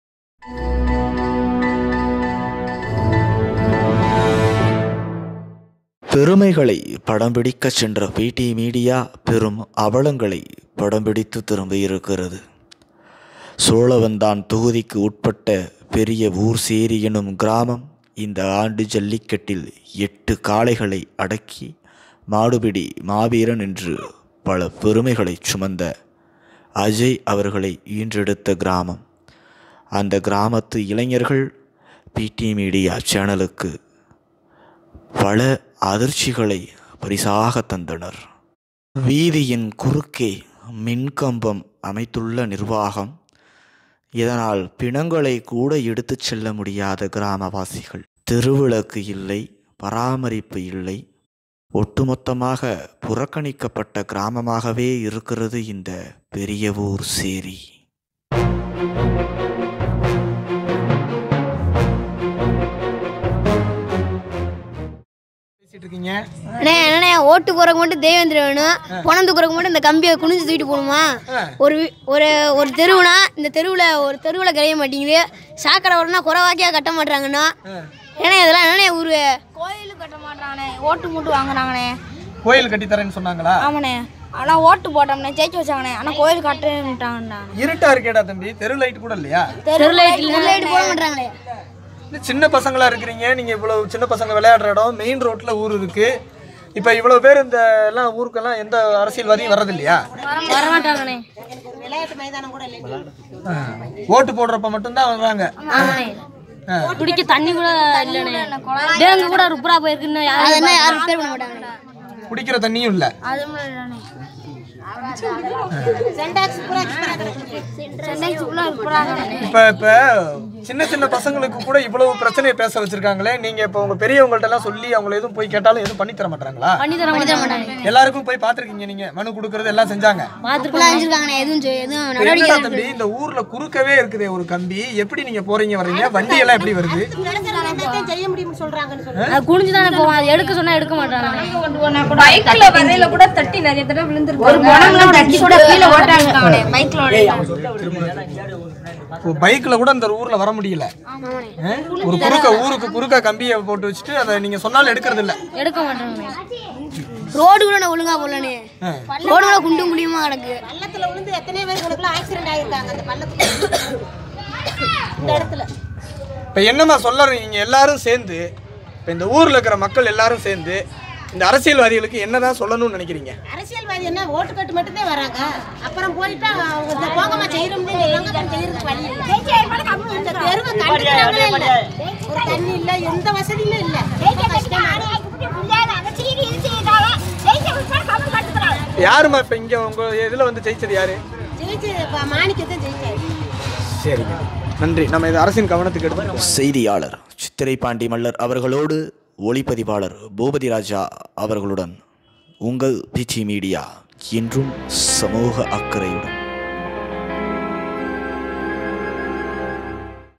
கதறும் சிறுவர்கள்,கிராம வாசிகள்! கதறவிட்ட MLA
மதுரை, சோழவந்தான் தொகுதியில் அமைந்துள்ள கிராமம் தான் பெரிய ஊர் சேரி.மாடுபிடி மாவீரன் அஜய் தேவேந்திரன் பிறந்த ஊர்தான் இது. அஜய் அவர்களது உறவினர்களை PT media சந்திக்கச் சென்ற போது ஊர் சிறுவர்களும் , முதியோர்களும் நம்மிடம் பெரும் சோகங்களை பகிர்ந்து கொண்டனர்.